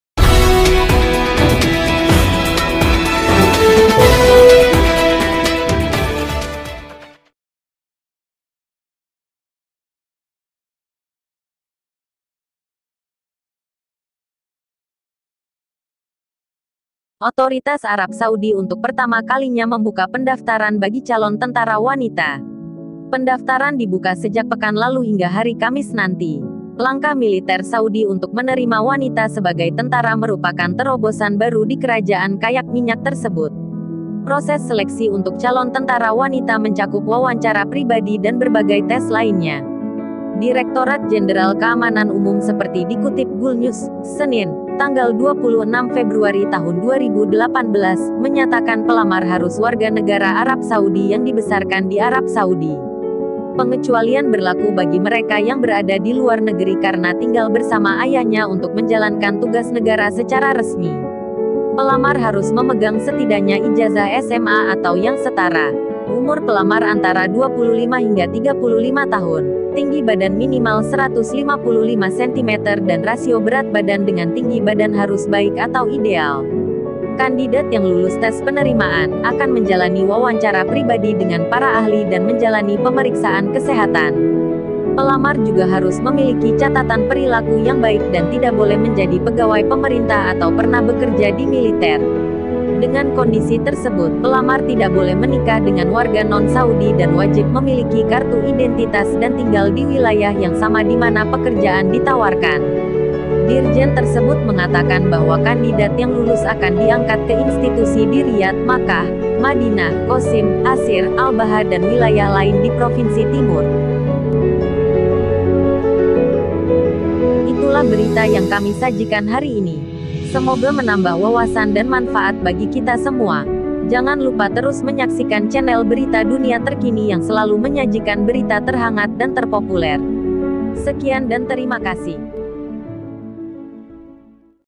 Pertama Kali, Wanita Arab Saudi Boleh Daftar Jadi Tentara Dengan Syarat ........
RIYADH - Otoritas Arab Saudi untuk pertama kalinya membuka pendaftaran bagi calon tentara wanita. Pendaftaran dibuka sejak pekan lalu hingga hari Kamis nanti.
Langkah militer Saudi untuk menerima wanita sebagai tentara merupakan terobosan baru di kerajaan kayak minyak tersebut.
Proses seleksi untuk calon tentara wanita mencakup wawancara pribadi dan berbagai tes lainnya.
Direktorat Jenderal Keamanan Umum seperti dikutip Gulfnews , Senin (26/2/2018), menyatakan pelamar harus warga negara Arab Saudi yang dibesarkan di Arab Saudi. Pengecualian berlaku bagi mereka yang berada di luar negeri karena tinggal bersama ayahnya untuk menjalankan tugas negara secara resmi.
Pelamar harus memegang setidaknya ijazah SMA atau yang setara. Umur pelamar antara 25 hingga 35 tahun, tinggi badan minimal 155 cm dan rasio berat badan dengan tinggi badan harus baik atau ideal.
Kandidat yang lulus tes penerimaan, akan menjalani wawancara pribadi dengan para ahli dan menjalani pemeriksaan kesehatan.
Pelamar juga harus memiliki catatan perilaku yang baik dan tidak boleh menjadi pegawai pemerintah atau pernah bekerja di militer.
Dengan kondisi tersebut, pelamar tidak boleh menikah dengan warga non-Saudi dan wajib memiliki kartu identitas dan tinggal di wilayah yang sama dimana pekerjaan ditawarkan.
Dirjen tersebut mengatakan bahwa kandidat yang lulus akan diangkat ke institusi di Riyadh, Makkah, Madinah, Qaseem, Aseer, Al Baha dan wilayah lain di provinsi timur.